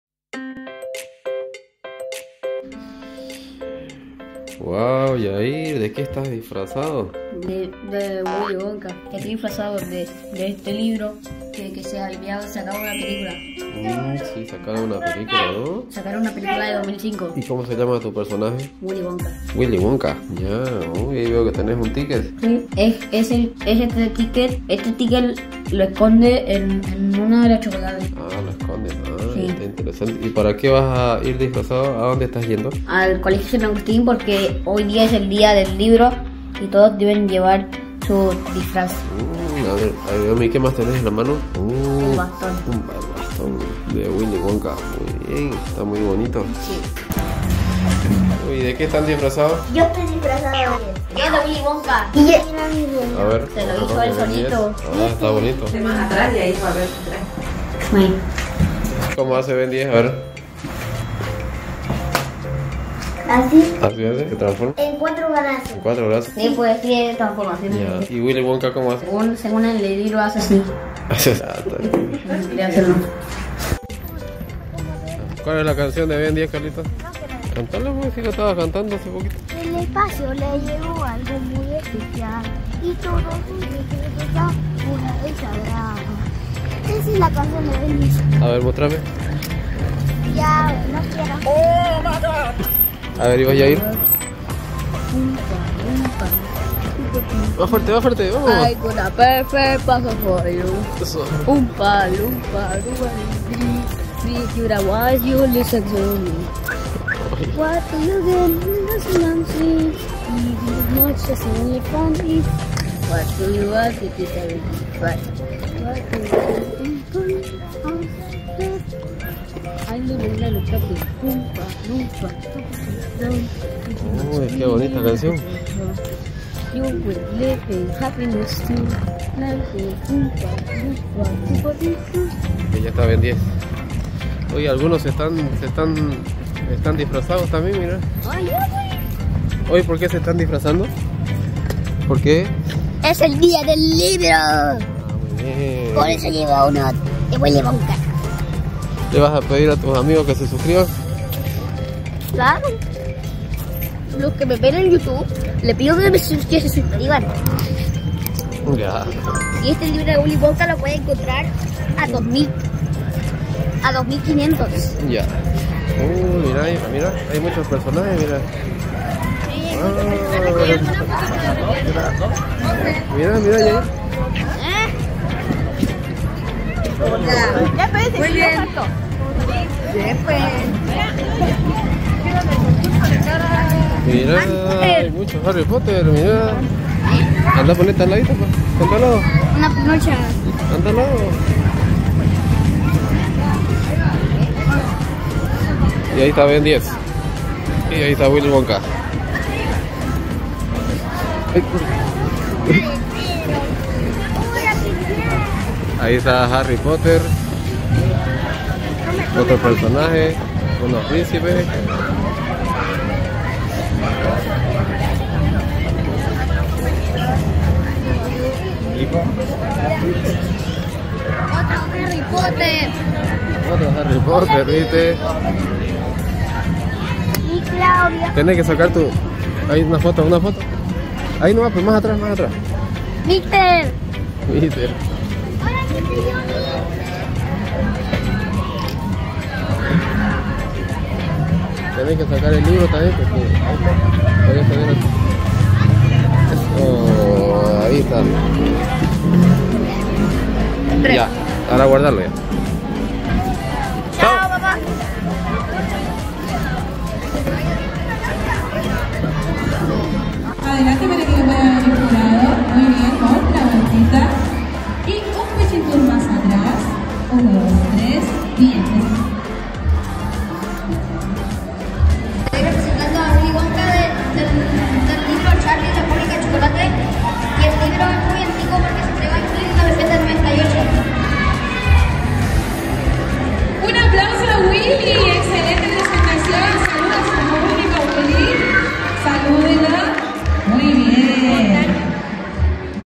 Mi Disfraz de Willy Wonka (Participé en un concurso)
En este video estoy disfrazado de Willy Wonka del libro Charlie y la Fábrica de Chocolates, debido a la Feria del Libro que se realizó en el  colegio San Agustín en Antofagasta.